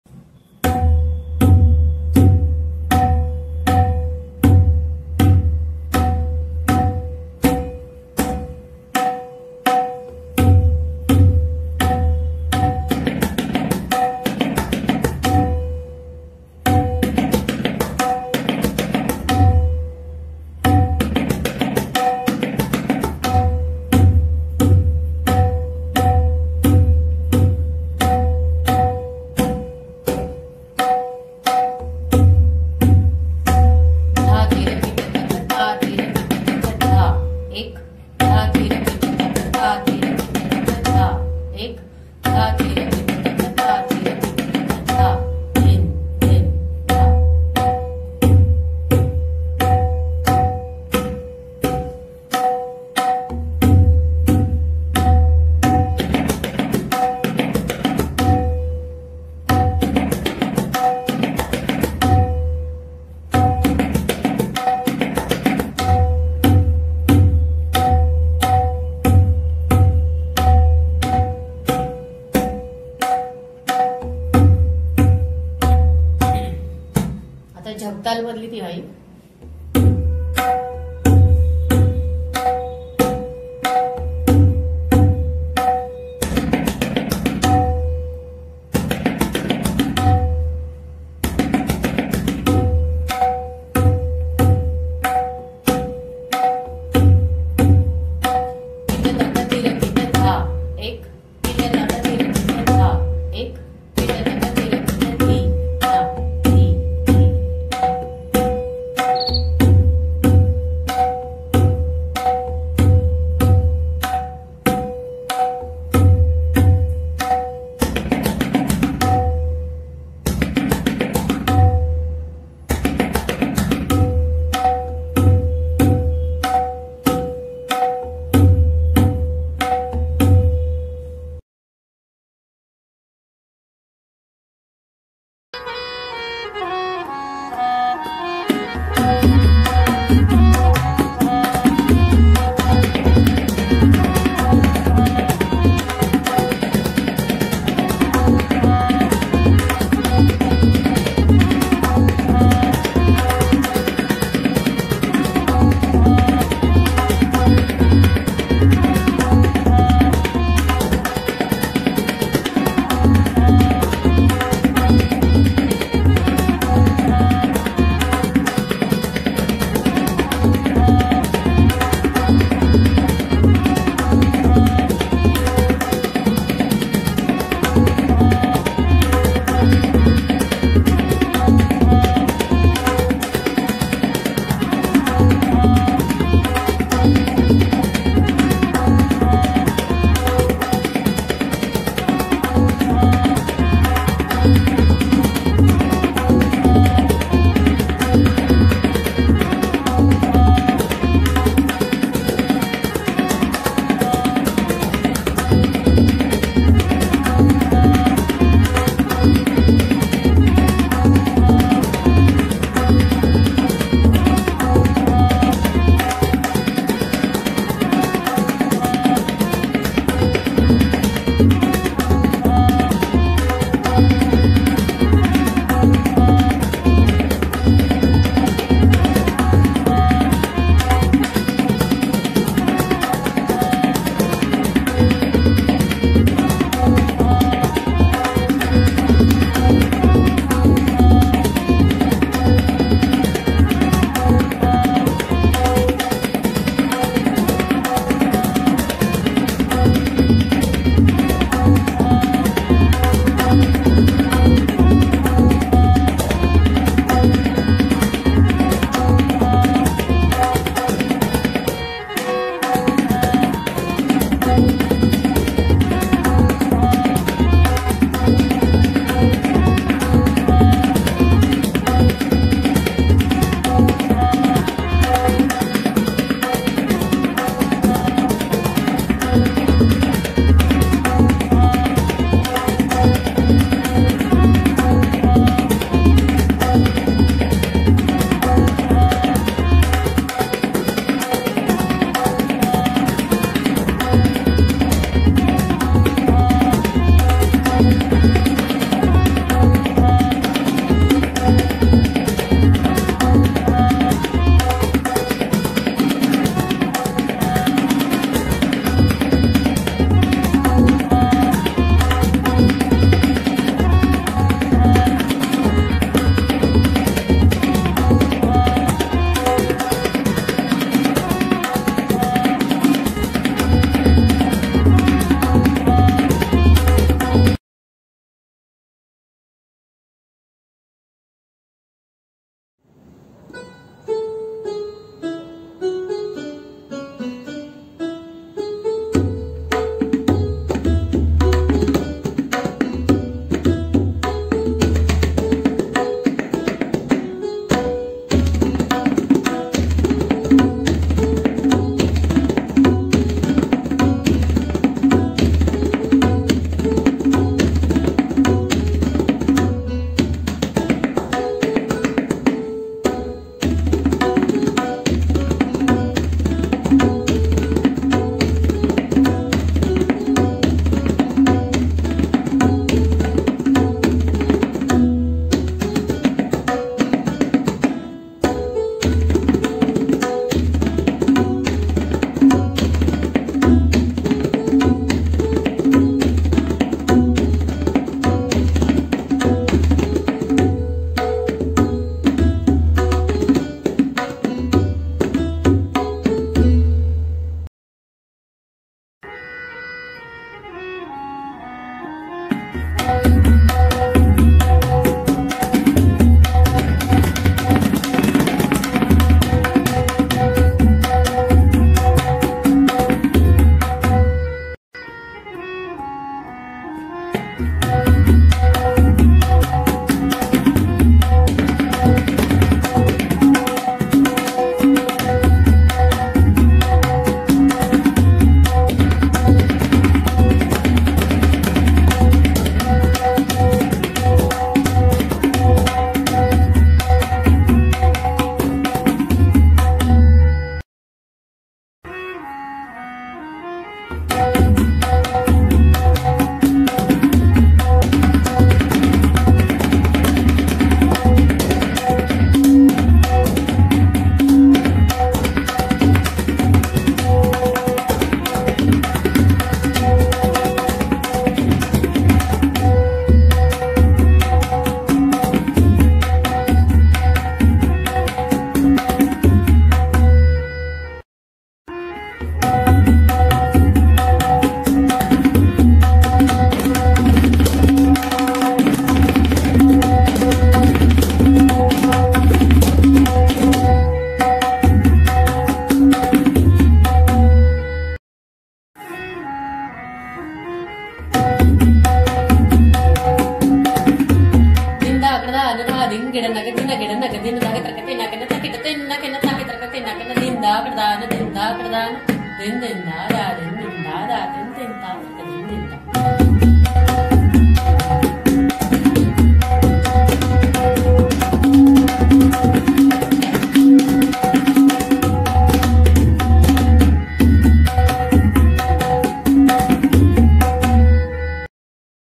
Tabla Vadan| Compilation of small videos from our YouTube channel
Compilation of YouTube videos and shorts from our YouTube channel. Tabla instructor: Mrunal Kulkarni.
Includes Zap taal, Teen taal etc.